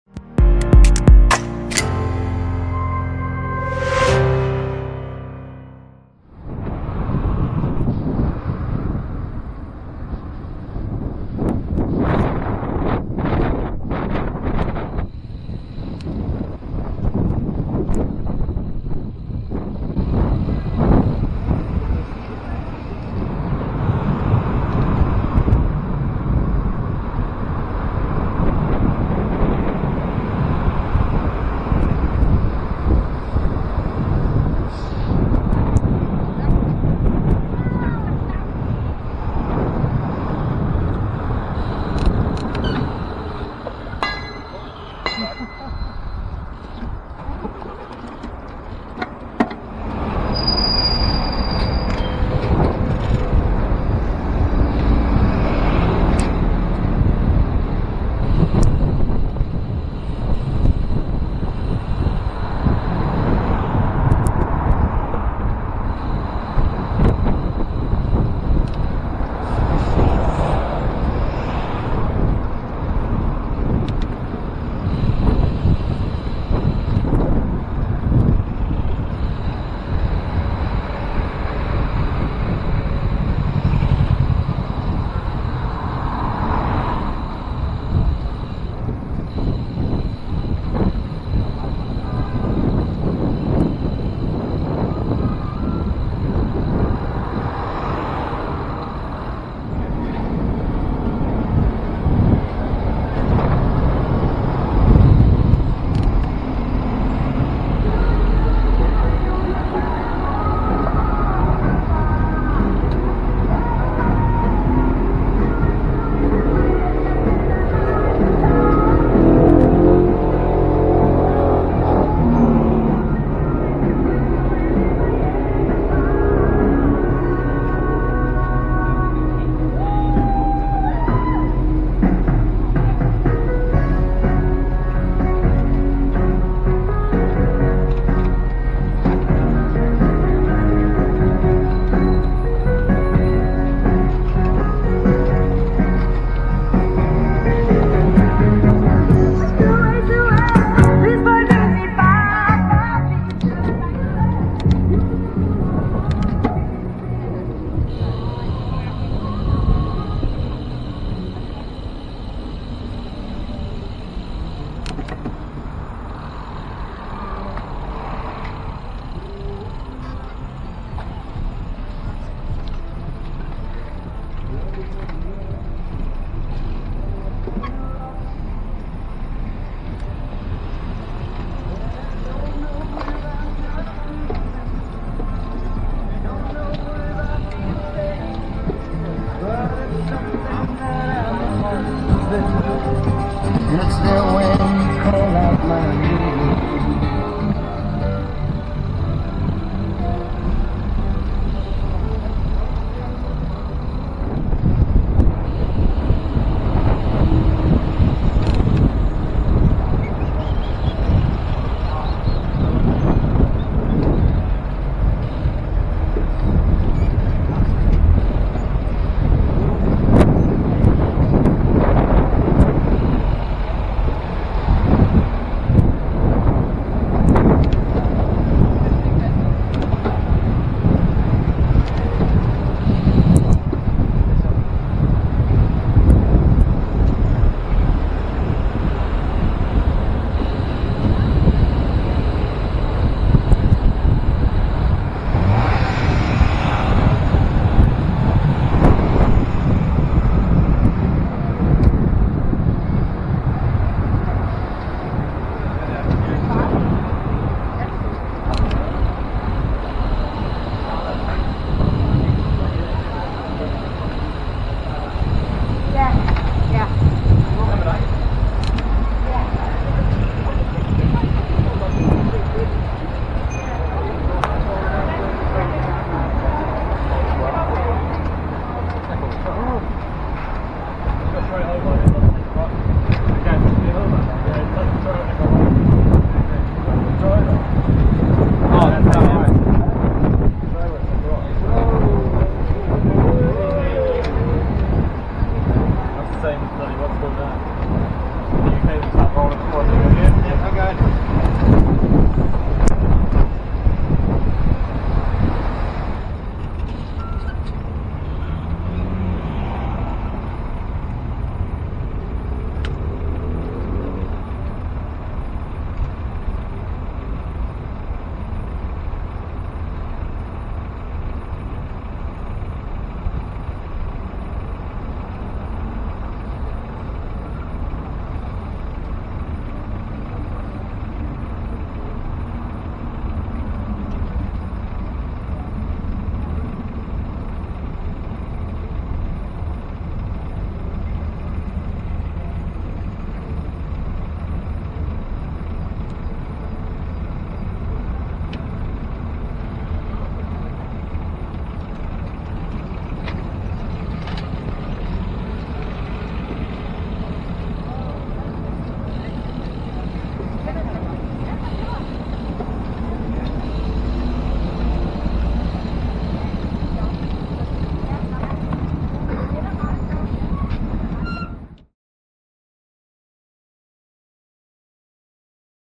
Cycling in Copenhagen #3: political campaigns, a Mannequin Challenge in rush hours and more!
(Turn sound ON)
On this episode, it's general elections time! And it's the season to watch the political parties doing their best to engage with the public. You will also see how people look weird when riding an electric scooter and we will finalize with a Mannequin Challenge during rush hours. Enjoy it!

List of events:
0:07 Master in Vertical Sleep
0:23 Hard Stop
0:50 Forget Lycra, Use Balloons Instead
1:40 We're Here To Cheer Your Commute
3:24 The Scooter Fraternity
5:06 Mannequin Challenge